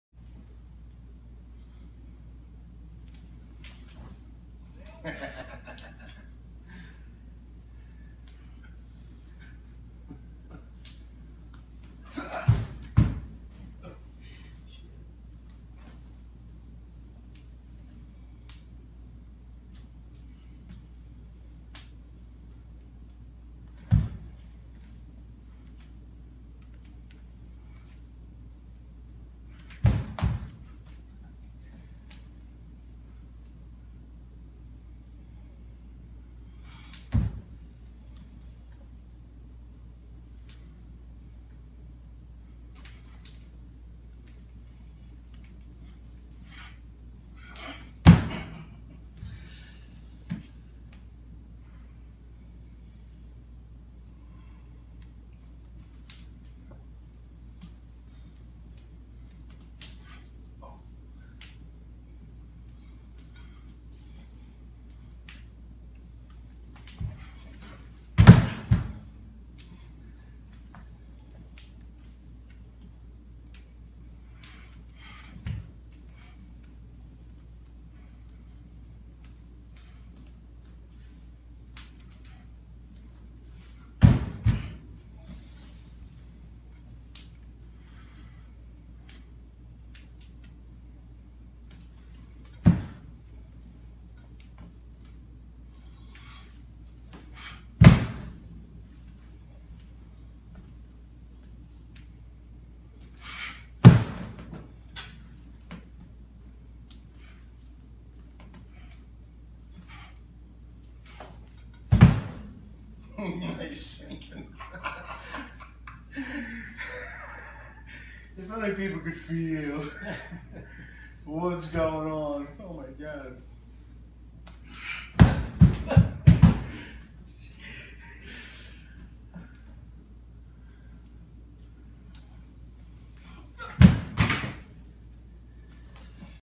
" Feeling is Believing "
In our modern age of technology many people sit behind a screen and observe the world from their homes . When I first got involved in the internal arts I was traumatized by the power of William CC Chen and Dr. Tao Ping Siang . I initially went to study these arts for health reasons as I had some personal health issues . I had studied other martial arts since I was a child and didn't even intend to study this for Martial purposes. Nothing you see with your eyes can prepare you with what you may feel with your body when getting involved in the internal martial arts. BP Chan used to say "feeling is believing". In this footage I use a variety of different methods to move Alex. He also sinks in the air while i am pushing him which makes me laugh as it's sort of a private joke between us. When I studied harder styles you could always pick out the athletes. Aside from the belts there was a swagger that they had that indicated you were going to get your ass kicked. When I first started getting involved in the Martial aspects of these arts, I was very startled by the various things that I felt. One of the great aspects about the internal arts is that they move counter to the natural degradations of the body that I experienced in other arts. The internal martial arts allows one to get stronger as they age and this may be their greatest value. If you have not experienced these types of arts then I recommend that you do as they will enrich your life.